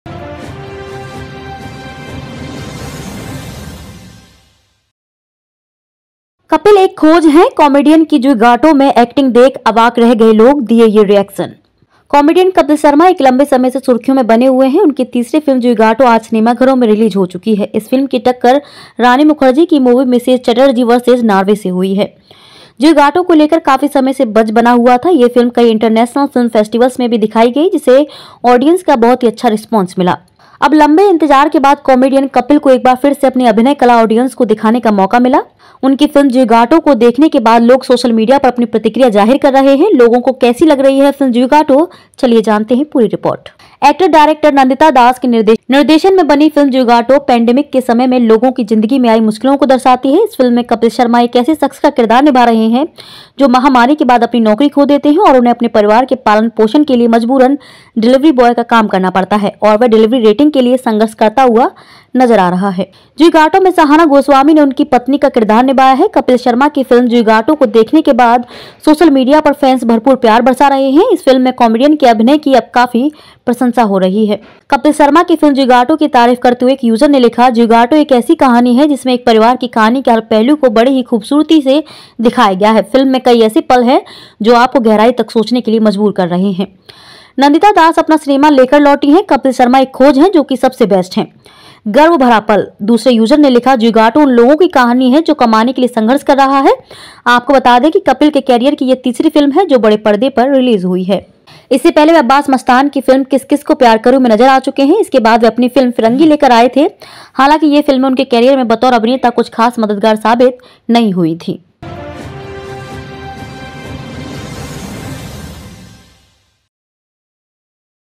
'कपिल एक खोज', कॉमेडियन की ज्विगाटो में एक्टिंग देख अवाक रह गए लोग,दिए ये रिएक्शन
Zwigato Twitter Review: कॉमेडियन कपिल शर्मा एक लंबे समय से सुर्खियों में बने हुए हैं। उनकी तीसरी फिल्म 'ज्विगाटो' आज सिनेमाघरों में रिलीज हो चुकी है। इस फिल्म की टक्कर रानी मुखर्जी की मूवी 'मिसेज चटर्जी वर्सेस नॉर्वे' से हुई है। ज्विगाटो को लेकर काफी समय से बज बना हुआ था। ये फिल्म कई इंटरनेशनल फिल्म फेस्टिवल्स में भी दिखाई गई, जिसे ऑडियंस का बहुत ही अच्छा रिस्पांस मिला। अब लंबे इंतजार के बाद कॉमेडियन कपिल को एक बार फिर से अपनी अभिनय कला ऑडियंस को दिखाने का मौका मिला।